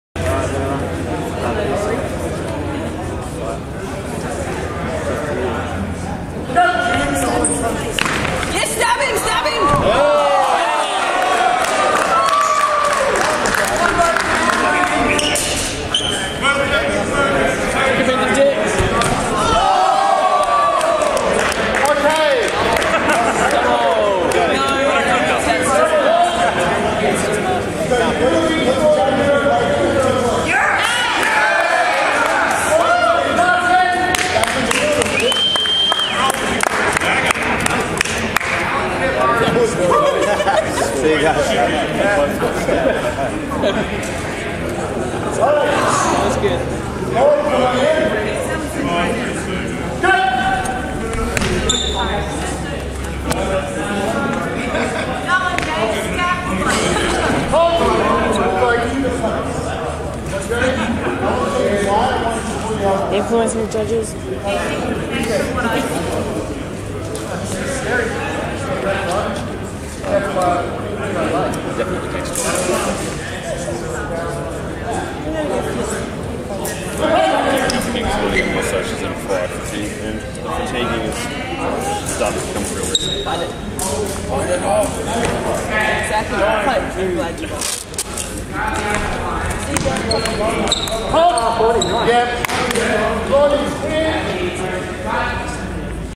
HDV 0053
Diverse footage from SwordPlay 2017, Brisbane Australia